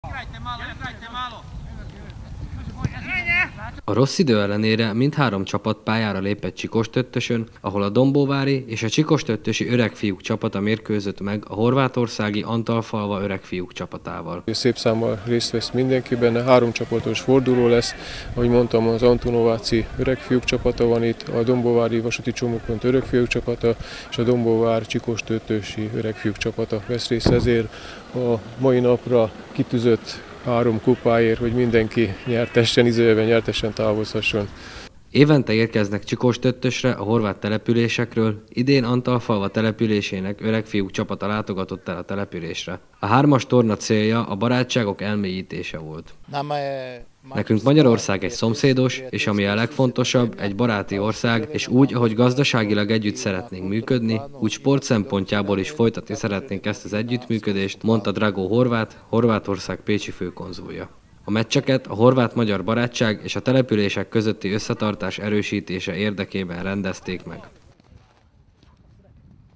Magyar-horvát foci meccset rendeztek Csikóstőttősön 20190415
Magyar-horvát meccset rendeztek Csikóstőttősön. Az eseményen a két nép közti barátságot erősítő találkozókat játszottak a csikóstőttősi, a dombóvári és a horvátországi Antalfalva öregfiúk csapatai.